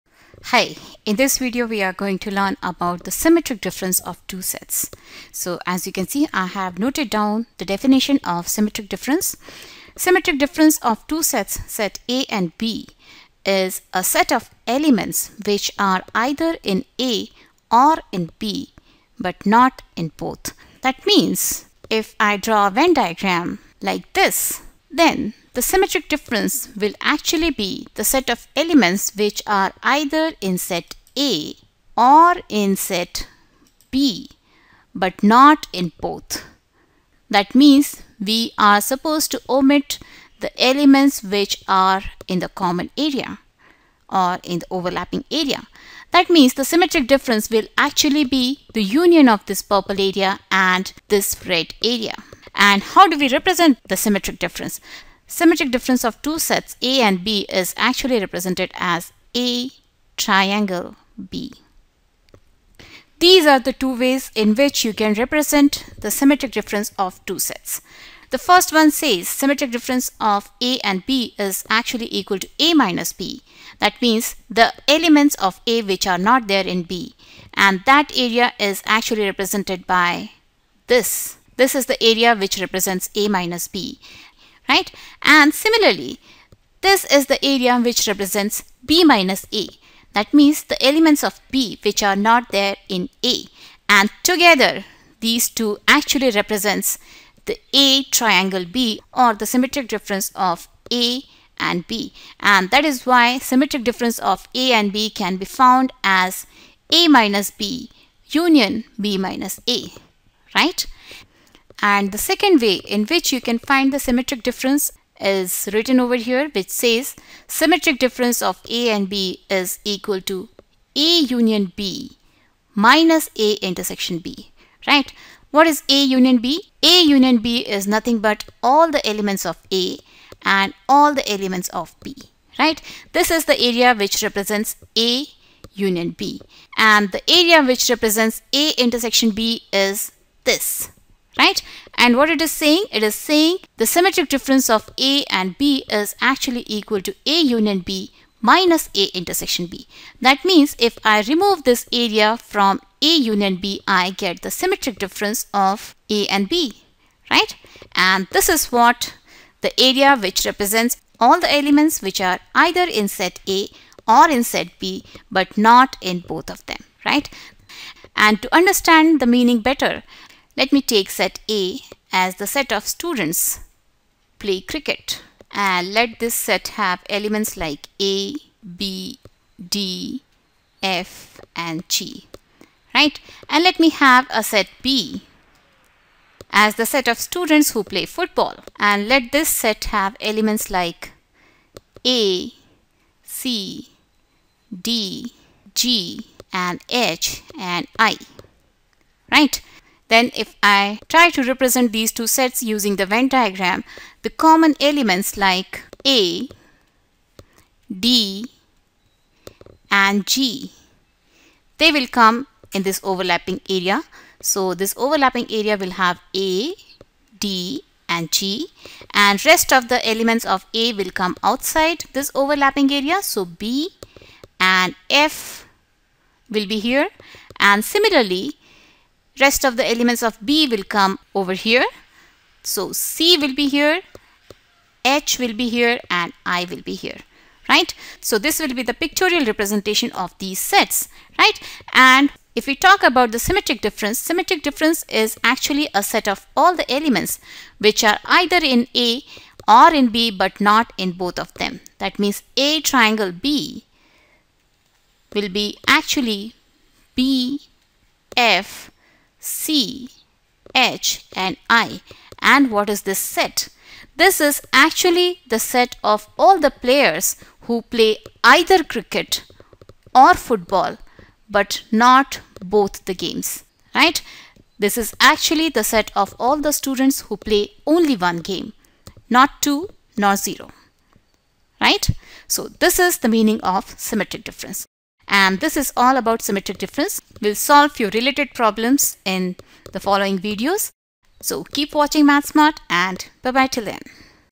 Set Theory : Symmetric difference of sets
Learn to find Symmetric difference of two sets. Also learn the meaning and usage of Symmetric difference.

Symmetric difference  of two sets A and B is the set of all elements that belong to either A or B but not  both A and B. It is the union of the sets (A - B) and ( B - A)